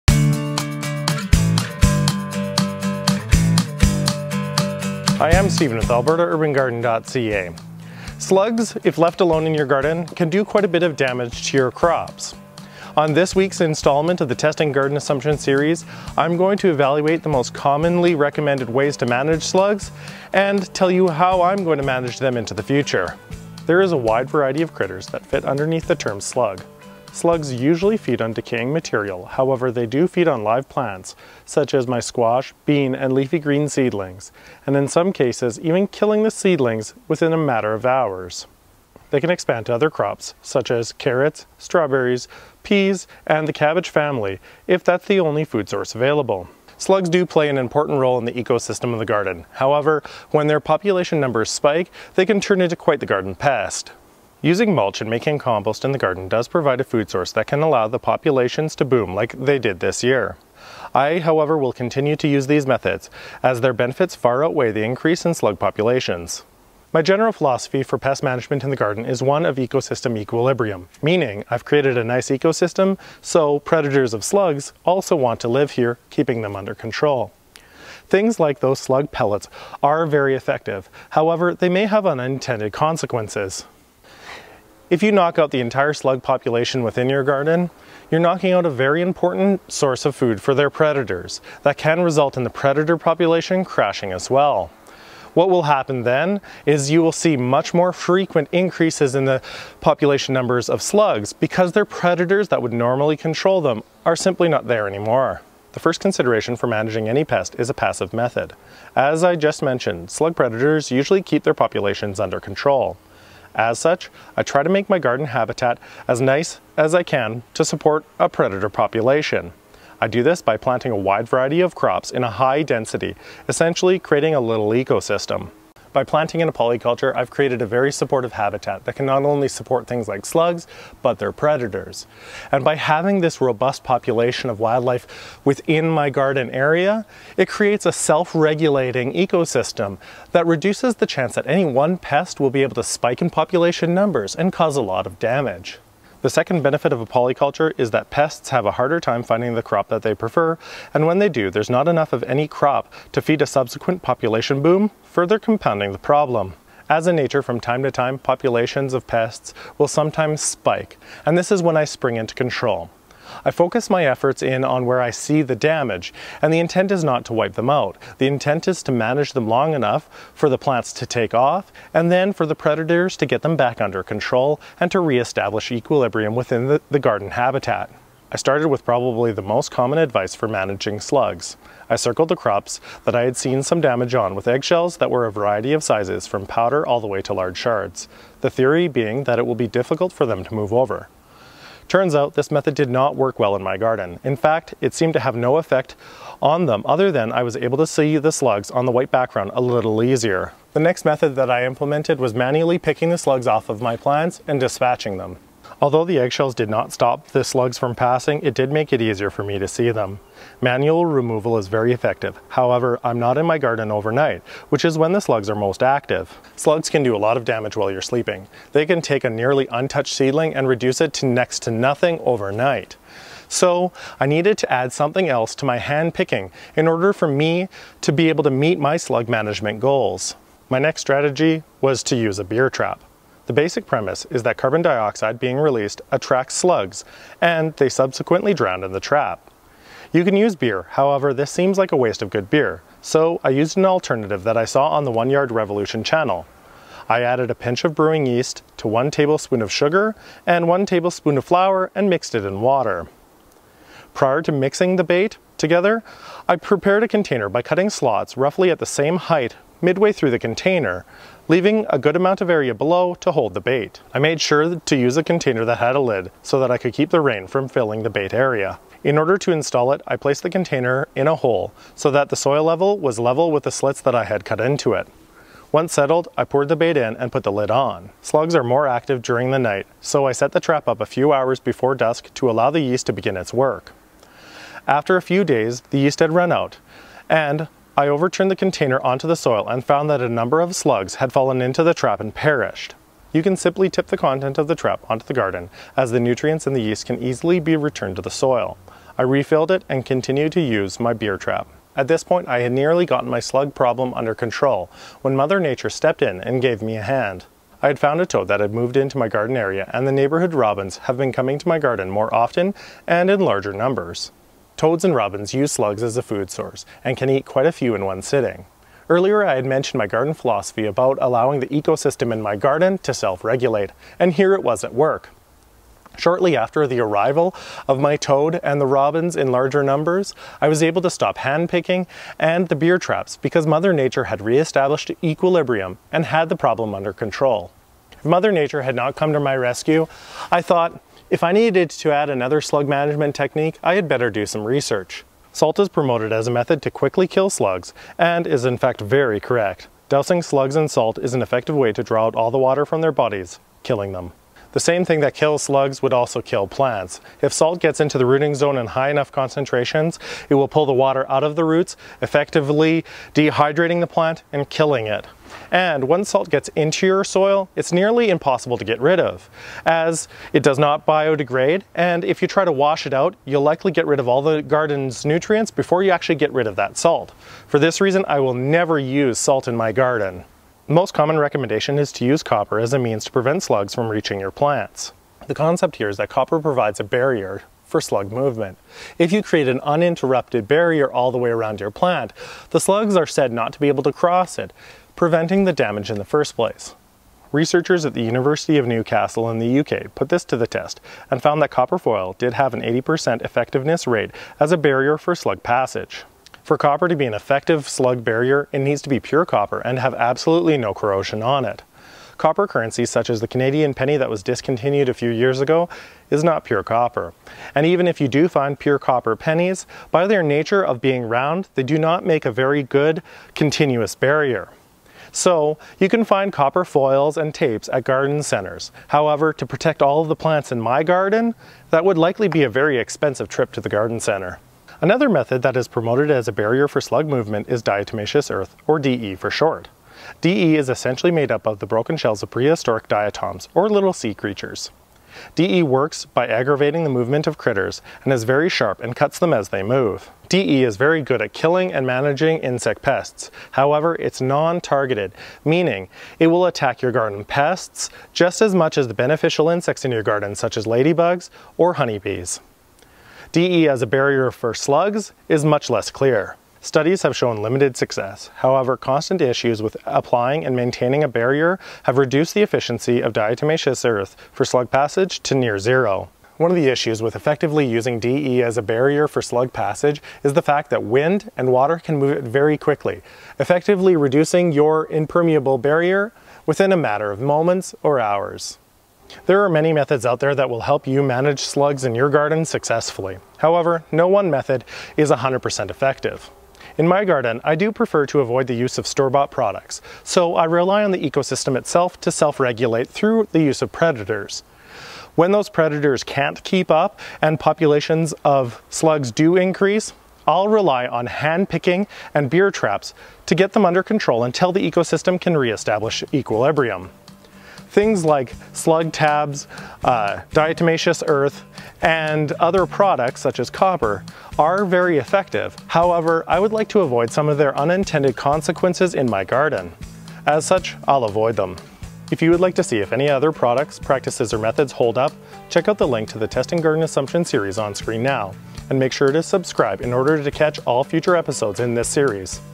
Does Organic Slug Control Really Work?
Slugs are garden pests that can do a lot of damage in a short period of time.  In today’s testing garden assumptions series video I am going to go through the most commonly recommended methods to manage slugs and how I am planning to manage them in my garden.

0:23 Are Slugs a Garden Pest?
1:06 General Pest Management Philosophy
1:45 Passive Slug Management
2:36 Active pest management
2:58 Do Eggshells Deter Slugs?
3:20 Manually picking and killing slugs
3:50 Beer Trap for Slug Management
5:01 Using predators to manage pests like slugs
5:45 Does Salt work to Deter slugs
6:20 Does Copper work to deter slugs
7:14 Is Diatomaceous earth effective for slug control